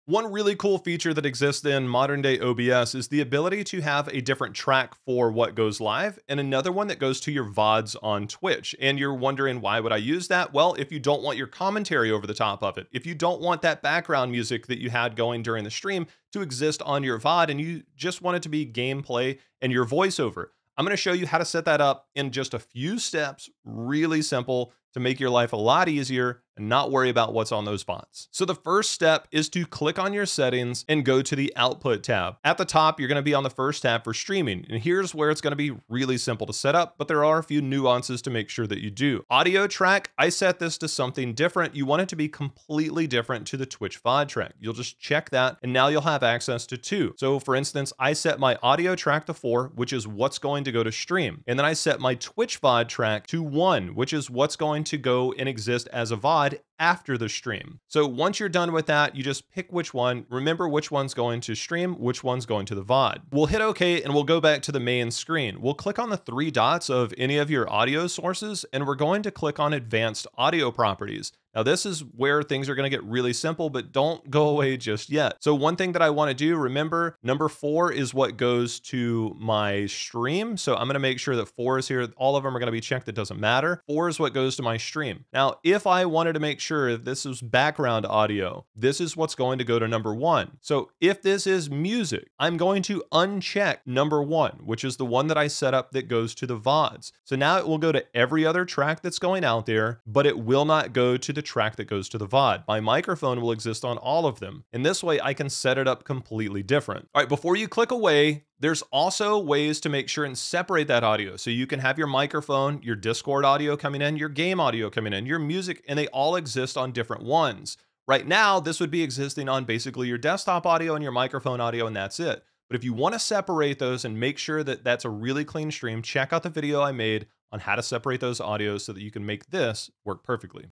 OBS How to Separate Twitch Audio (BETTER GUIDE THAN OTHERS)
This video will show you how to separate audio on Twitch! Using OBS to separate audio tracks is super simple to separate music from vods. You can also use this to separate discord audio as well :)

Setting up a Twitch VOD track in OBS Studio is one of the best ways to produce content for VODs on Twitch, YouTube content, or clips to publish elsewhere. Having background music is great, but it doesn't always help content after the fact.

Separating the audio and then choosing which track will help you record and stream content that people can watch long after the stream is done without all the discord audio, mics, music, or stream alerts. Whatever you want, you have complete control over the Twitch VOD track with a few easy OBS settings in this tutorial.